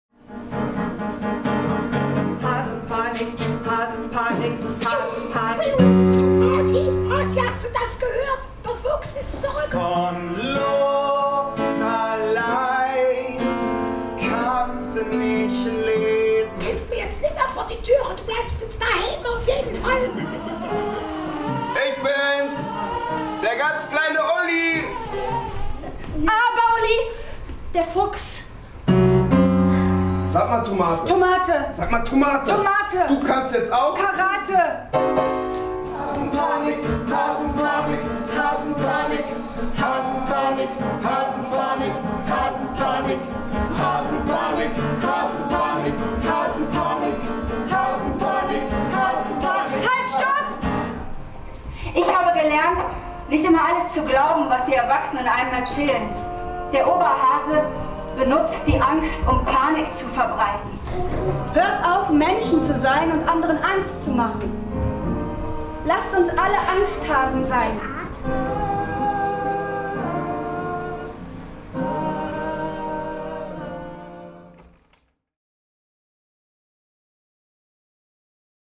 Der kleine Angsthase — von Elizabeth Shaw — Junges Schauspiel Düsseldorf
Der kleine Angsthase — nach dem Bilderbuch von Elizabeth Shaw — Für alle ab 4 Jahren — Premiere am 6. Januar 2018 im Jungen Schauspiel Düsseldorf

Der kleine Angsthase hat vor allem Angst: vor Hunden, vor Räubern, vor der Dunkelheit und vor Wasser, denn davor hatte ihn die liebe, aber ebenso ängstliche Hasenoma gewarnt. Der kleine Angsthase fürchtet sich sogar vor den größeren Hasenkindern. Die sind besonders beängstigend, findet er. Hört er sie »Angsthase, Angsthase« rufen, legt er schnell die Löffel an und wäre am liebsten unsichtbar. Da helfen auch die guten Ratschläge von Onkel Heinrich nicht: »Du musst deine Angst überwinden, sei einfach nicht mehr ängstlich!« Aber wie soll das gehen? Nur der ganz kleine Ulli, der noch viel kleiner ist als er selbst, scheint ziemlich ungefährlich. So spielen die beiden Hasen mitei nander, bis eines Tages der Fuchs ins Dorf geschlichen kommt. Viel zu klein, um schnell wegzuhoppeln, sitzt Ulli in der Falle. Was soll der kleine Angsthase bloß tun? Er kann nur noch einen klaren Gedanken fassen: »Der böse Fuchs frisst meinen ganz kleinen Ulli.« Da nimmt der kleine Angsthase all seinen Mut zusammen.